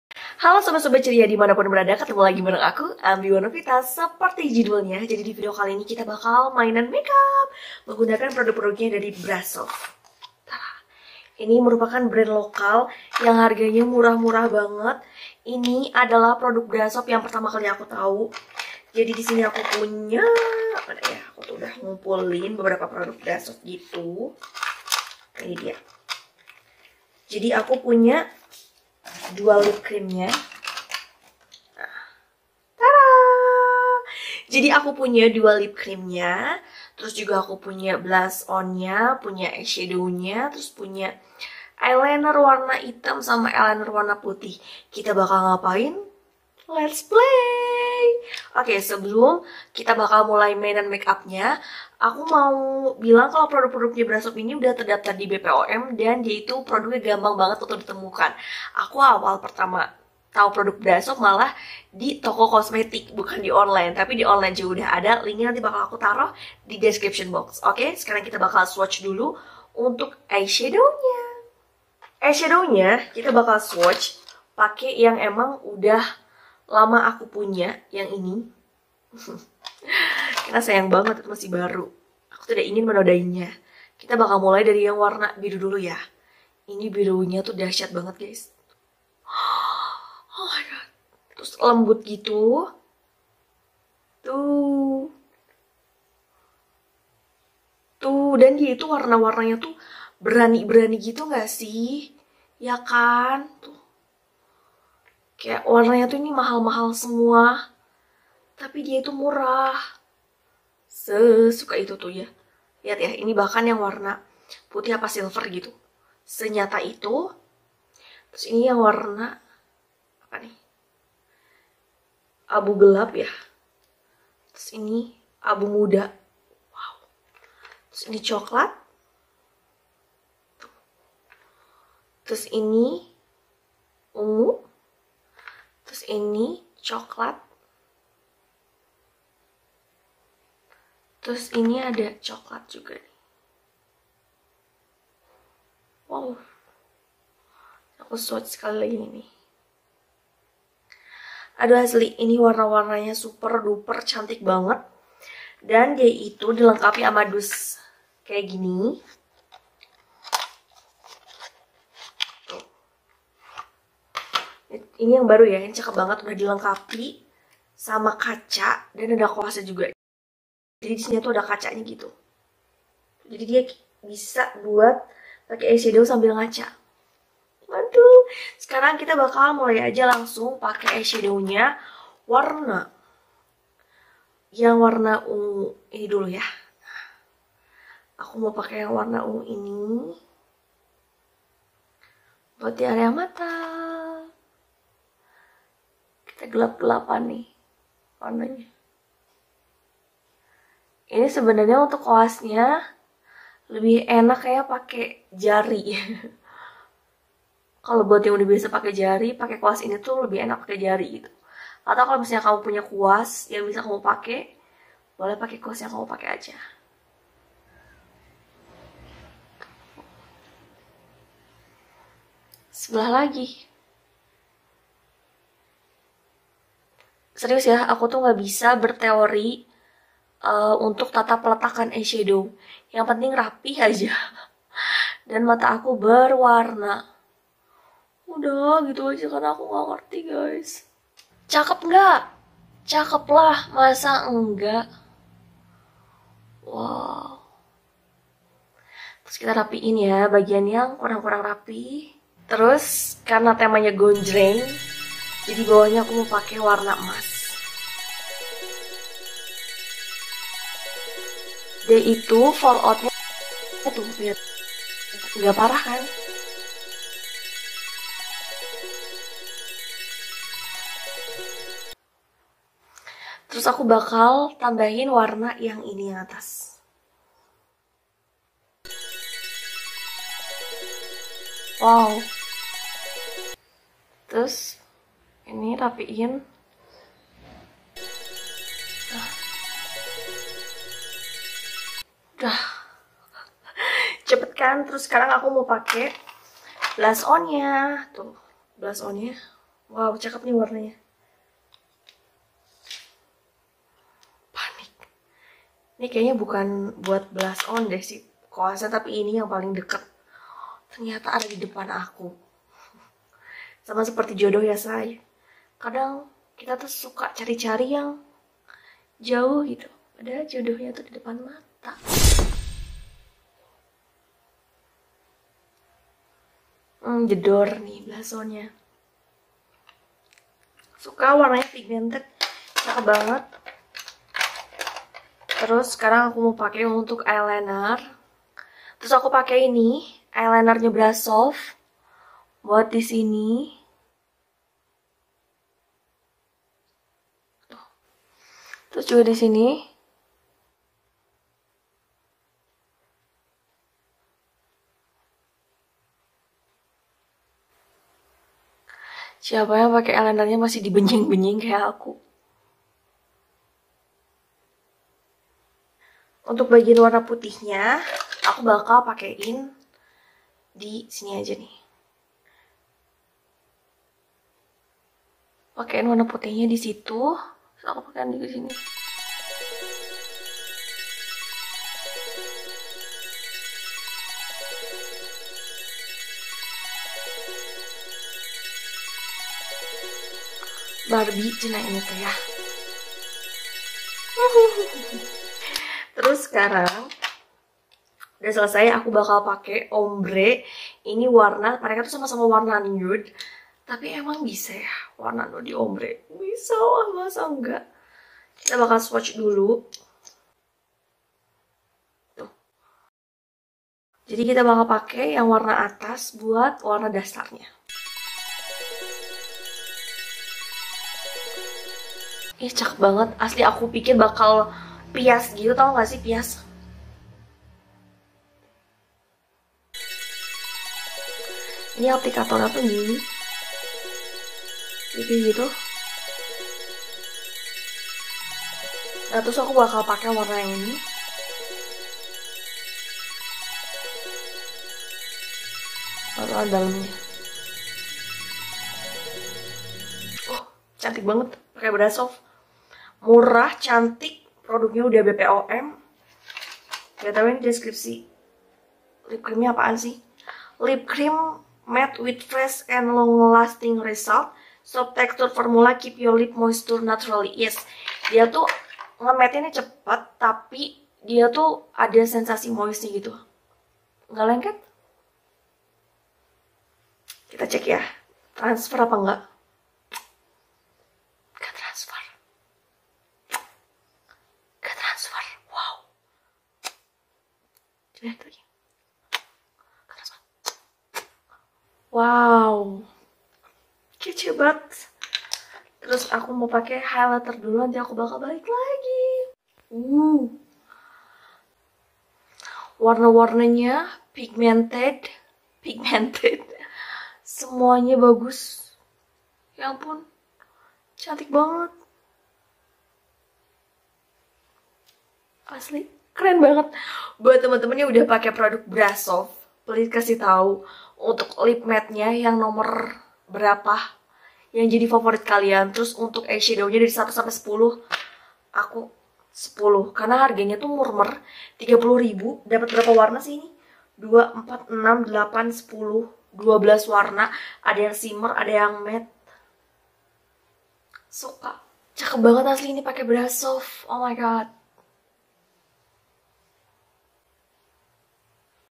Purple Glam Make Up BRASOV Murah Banget Bagus Banget!
mau nangis ga sih punya produk make up sebagus ini😭 dan murah -murah gais!!

2. Brasov Eyeliner Putih

4. Brasov Lip Cream Matte

5. Brasov eyeliner hitam

+ FYI :

Tipe kulit : Normal

📸 :    sony xperia x performance